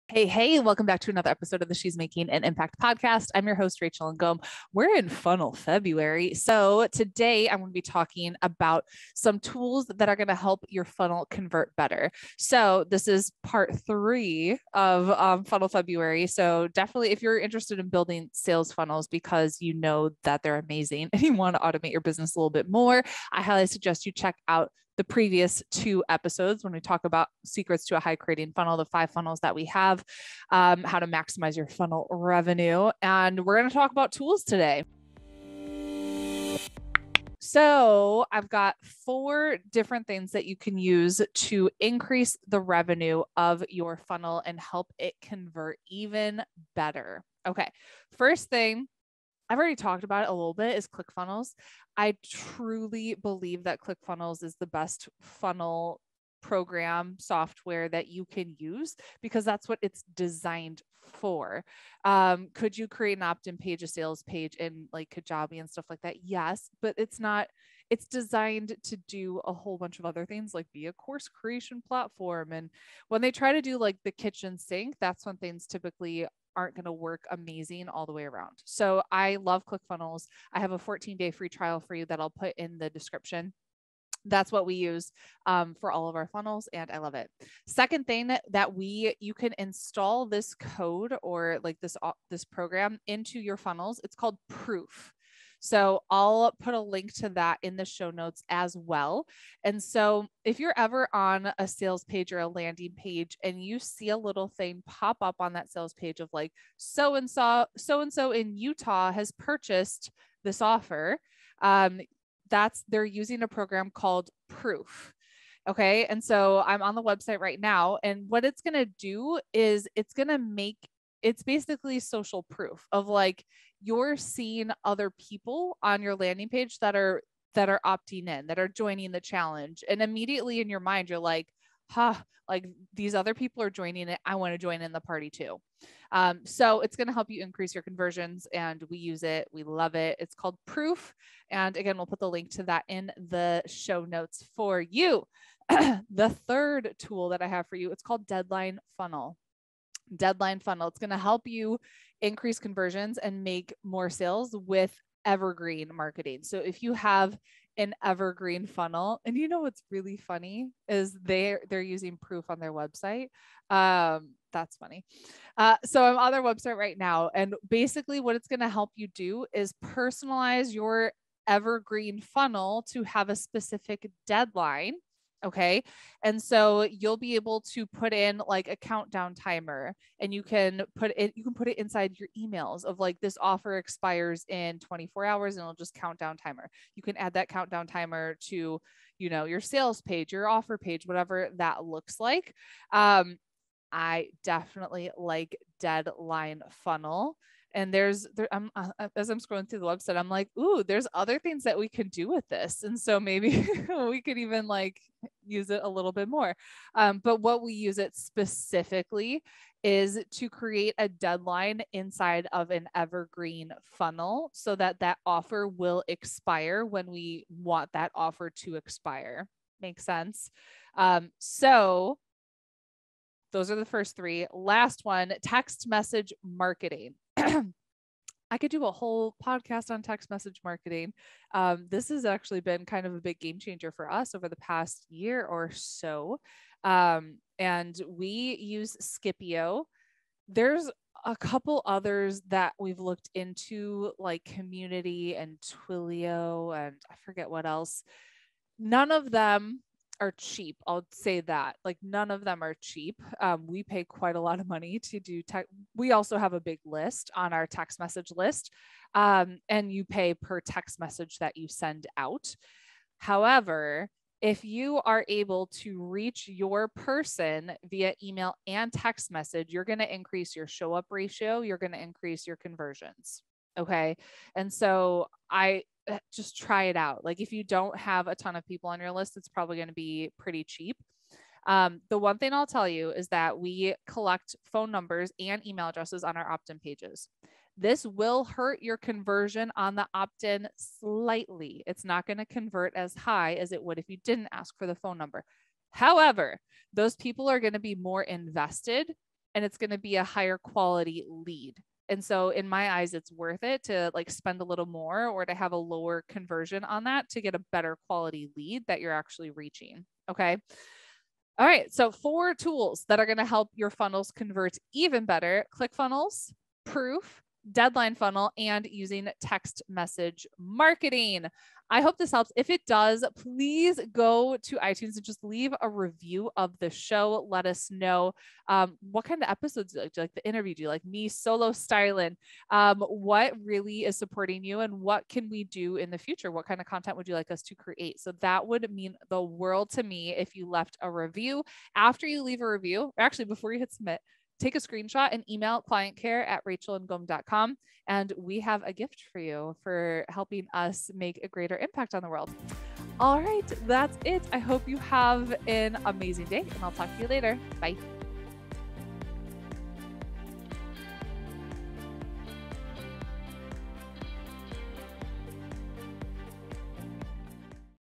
Tools to Help Your Funnel Convert Better | Rachel Ngom
Video description:
It's Funnel February!! And in this week's video, I want to share the tools we use within our funnel to help it convert better! Join me every Monday of February where I’m diving into funnels, and how you can start to see so much growth by implementing my funnel tips!!